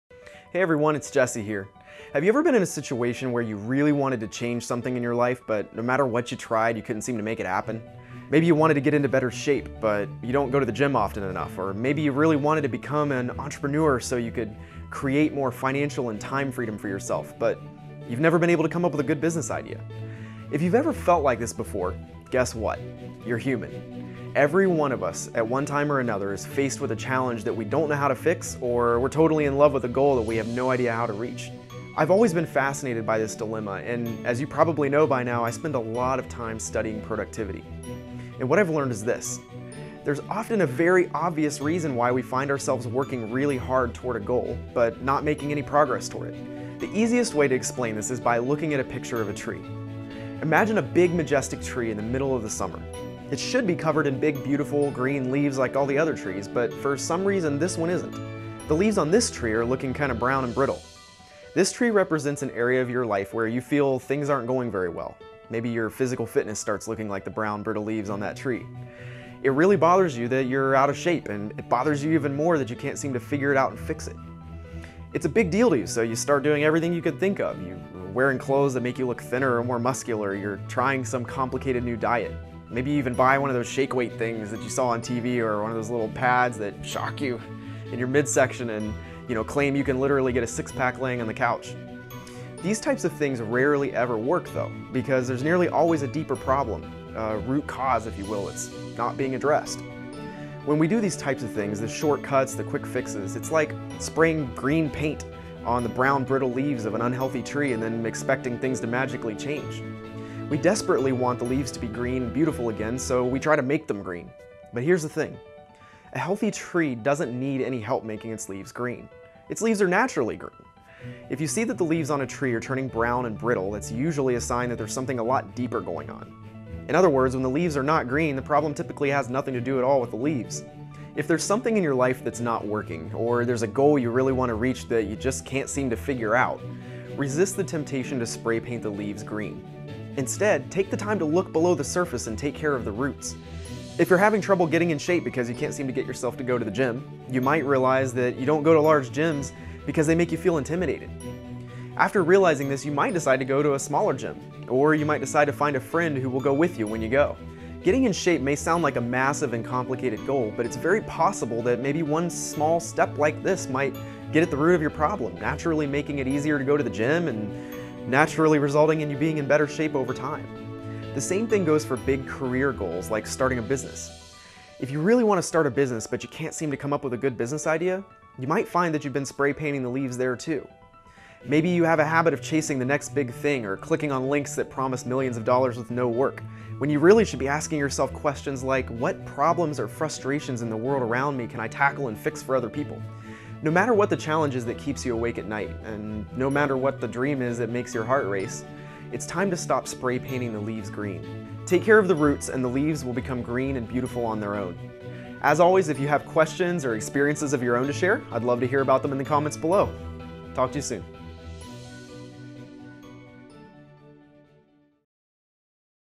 Why Most People Never Reach Their Goals
Have you ever struggled to reach a goal and wondered why nothing you try seems to work? That's exactly how I used to feel, but here's what I learned...